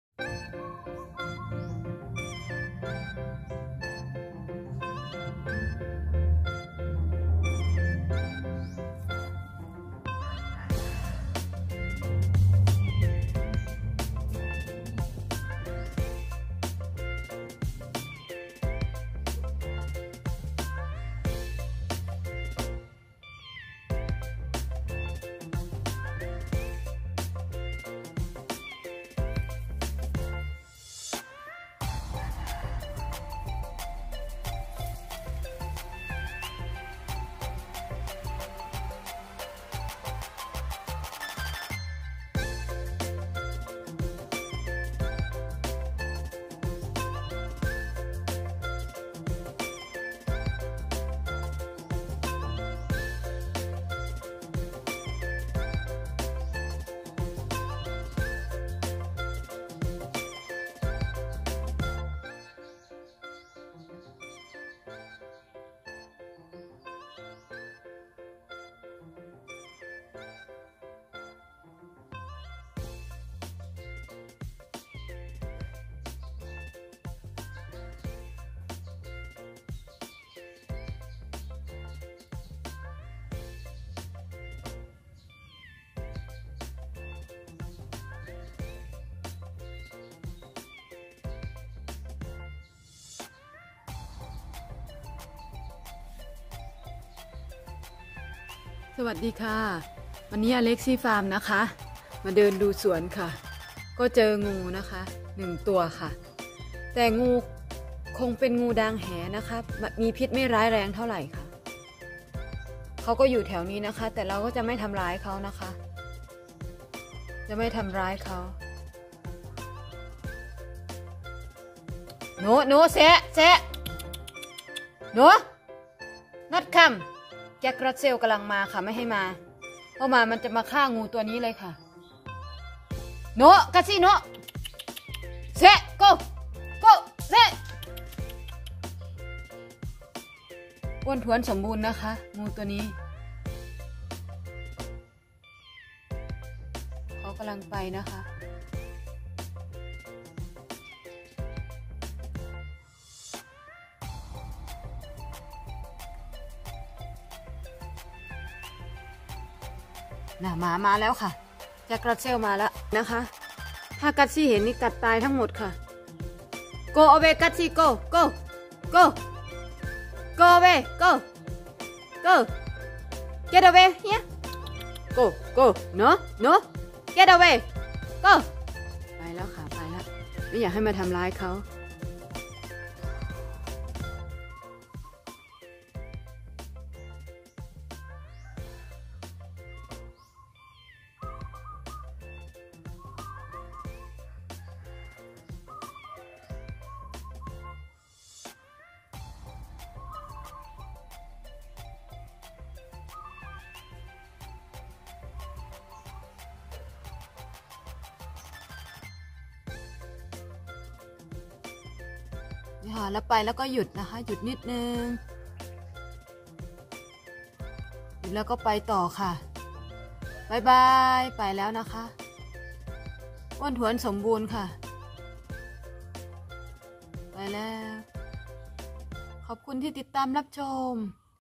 งูในสวน
งูลายสอบ้านหรือคนอิสานบ้านเฮาเรียกงูดางแหในสวนช่วยจับหนูและไม่มีพิษ